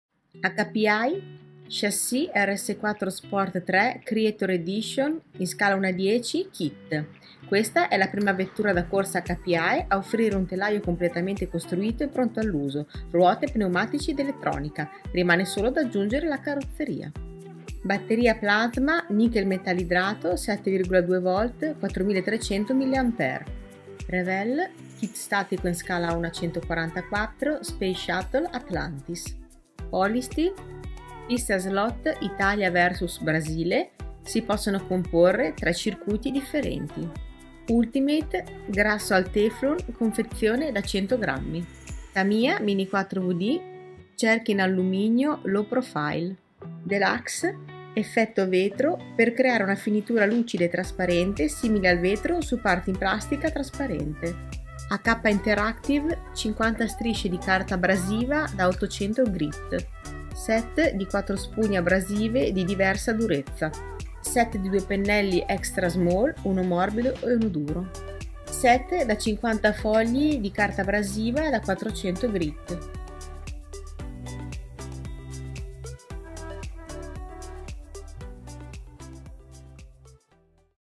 News Modellismo #69
Una breve rassegna delle novità arrivate o prodotti interessanti riassortiti a magazzino.

- Pista Mundial 3 in 1 Italia VS Brasile in 3 circuiti diversi Polistil